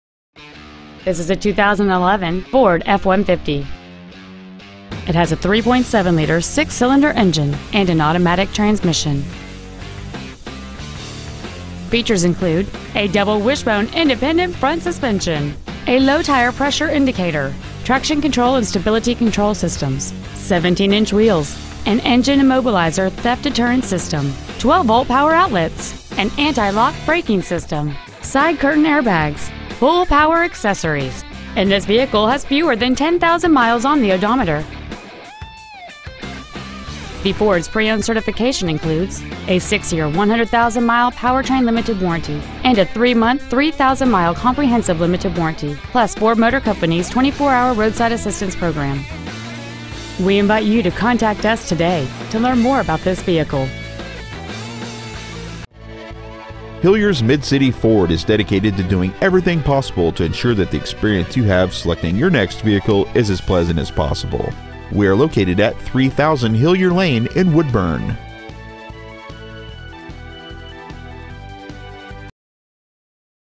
2011 Ford F-150 Certified Woodburn OR
We have been honored to serve the Woodburn OR area , we promise that your experience at our dealership will exceed your expectations !

 Year : 2011
 Make : Ford
 Model : F-150
 Engine : 3.7L V6 24V MPFI DOHC FLEXIBLE FUEL
 Trans . : 6-SPEED AUTOMATIC
 Exterior : BLUE
 Miles : 9,705
 Interior : GRAY
 Stock : PC1192

 3000 Newberg Highway
 Woodburn , OR 97071